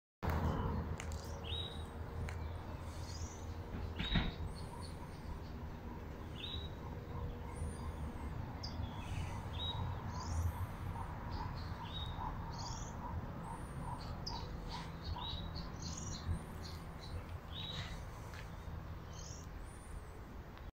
Our fur babies soaking up the morning sun.
Our Pekingese dogs enjoying the morning sun after days of rain.
Most common reasons for them laying in the sun.
Source of body heat.
As an antidepressant.
A source of vitamin D.
A sleep aid.
Build their immune systems.